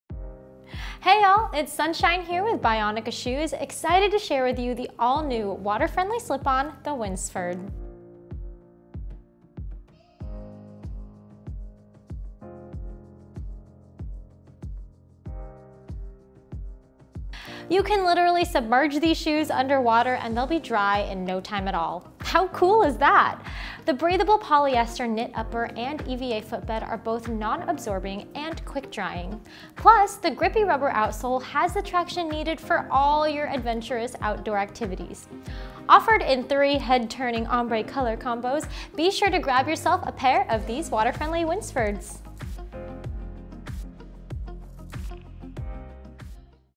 Bionica Shoes - the WINSFORD
The Winsford's water-friendly and breathable engineered knit upper has just the right amount of stretch to hug the foot - while turning heads with its sporty eye-catching ombre color combinations! The sporty, outdoor-ready outsole and quick-drying, water-friendly footbed team up to bring you non-stop comfort for your next outdoor excursion!